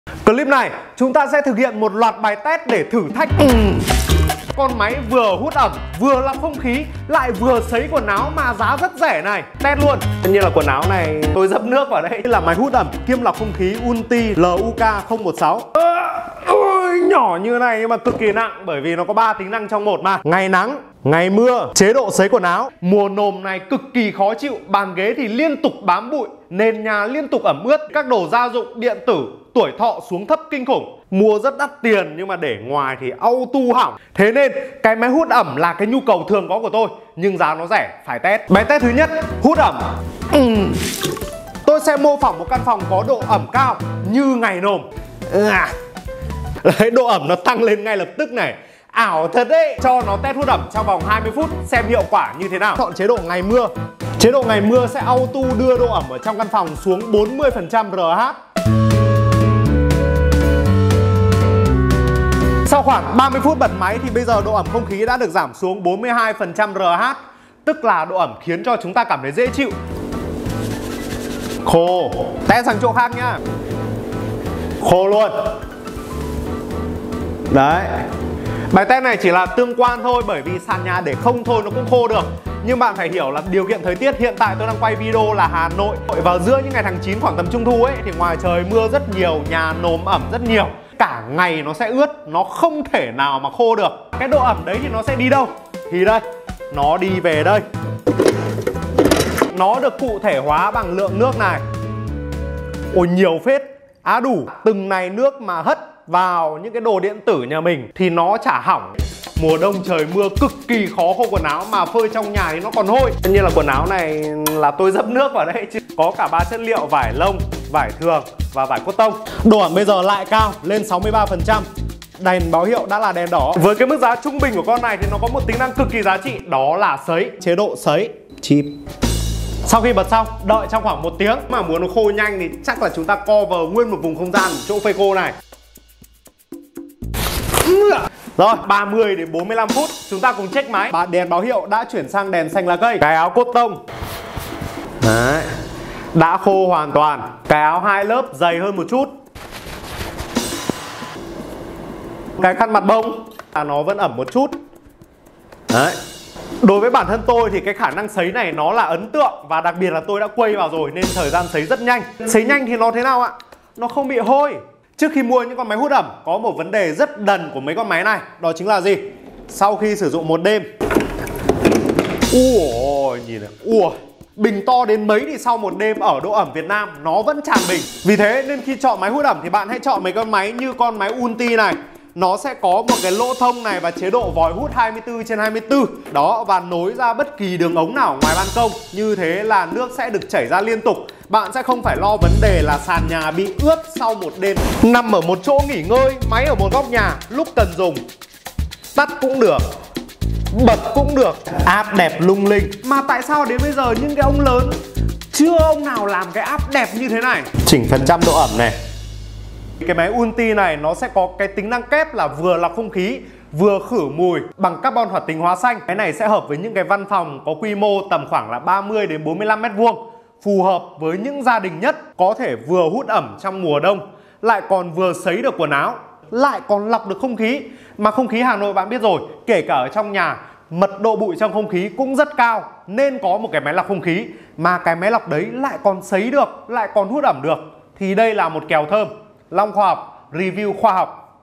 Long Khoa Học review SIÊU PHẨM khắc chế thời tiết Nồm Ẩm | áy hút ẩm -lọc không khí ULTTY LUK016
Long Khoa Học và SIÊU PHẨM KHẮC CHẾ trời Nồm Ẩm | áy hút ẩm - lọc không khí ULTTY LUK016